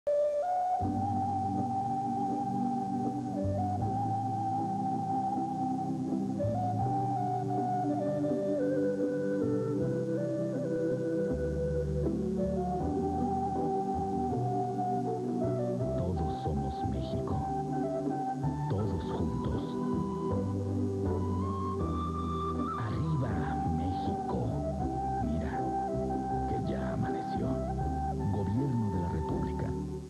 Comerciales mexicanos: Gobierno de la Republica 2000 (gobierno de Vicente Fox)
Uno de los primeros spots publicitarios cuando recién gobernaba Vicente Fox a finales del 2000 "Todos somos México"